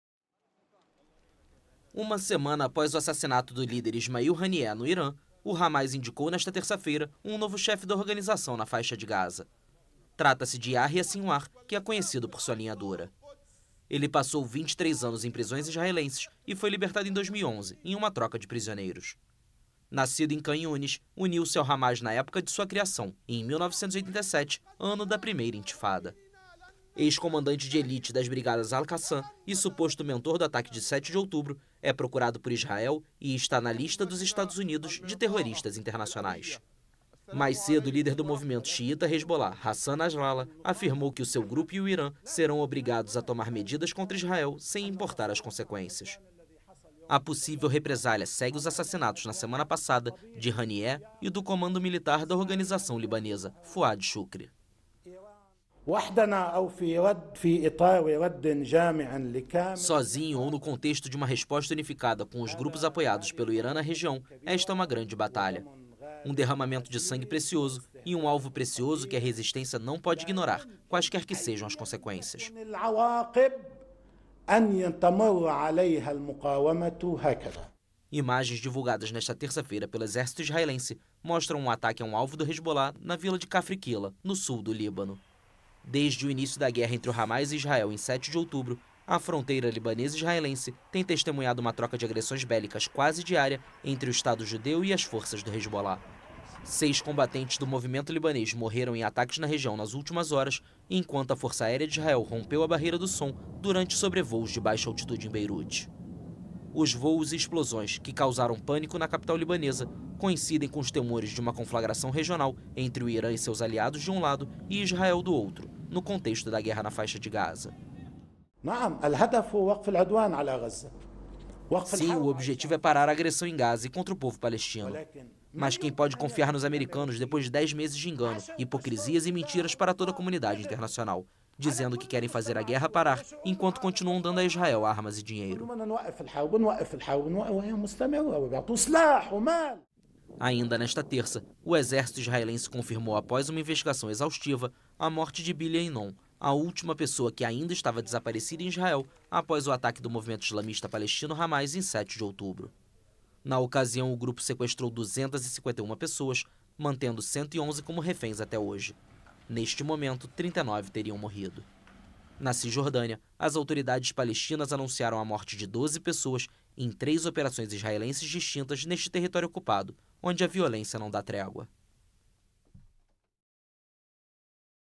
Hamas nomeia Yahya Sinwar como líder político para substituir Haniyeh | AFP
O movimento islamista palestino Hamas anunciou, nesta terça-feira (6), que nomeou Yahya Sinwar, chefe da organização na Faixa de Gaza, como seu novo líder político.